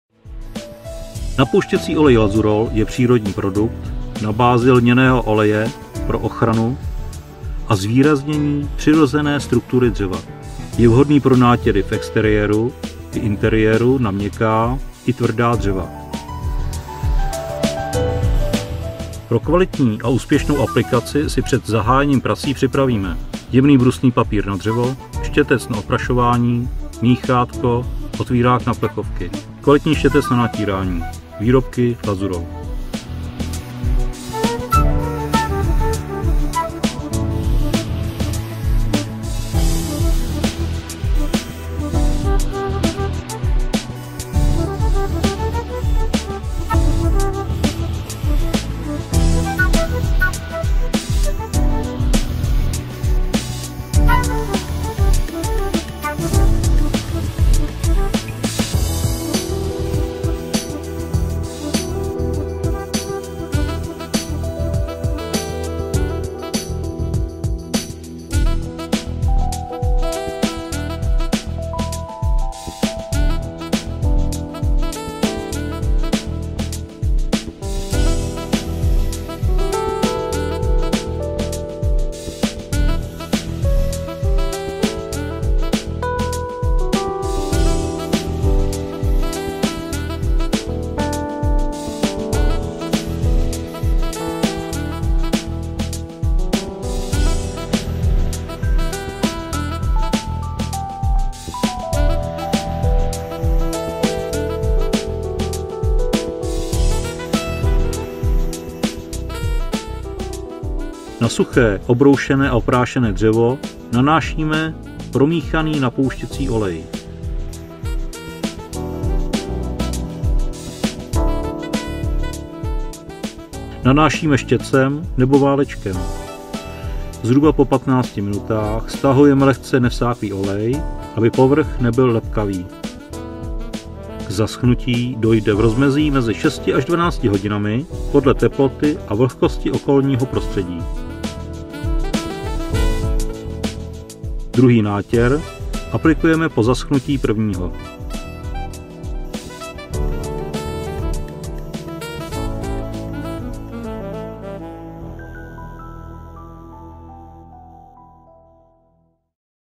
Ako ošetrit záhradný nábytok?
Nedovoľ počasiu zničiť tvoj záhradný nábytok. Návod ako správne ošetriť záhradný nábytok nájdeš  v tomto krátkom inštruktážnom videu. Použite správny olej na drevo.